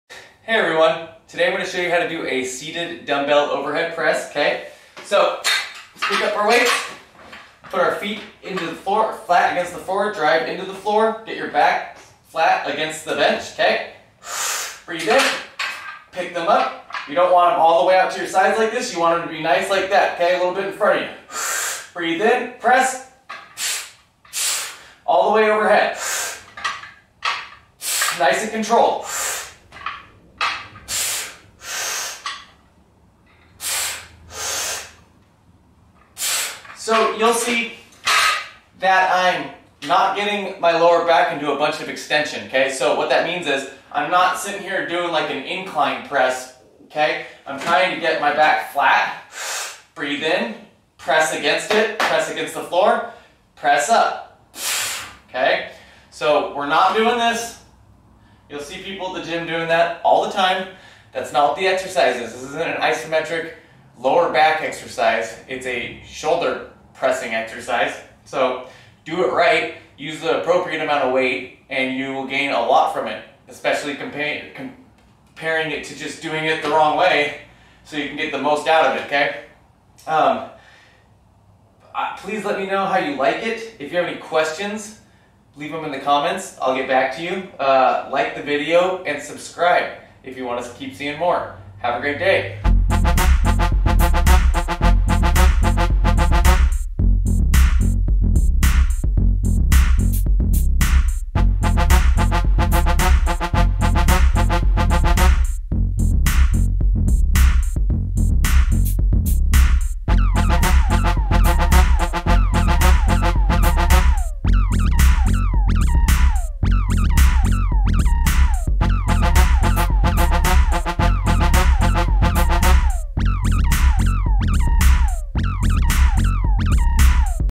Seated Dumbbell Overhead Press Exercise Tutorial - Sound Science Strength
Sound Science Strength

Straight to the point exercise tutorials

How to do a seated dumbbell overhead press exercise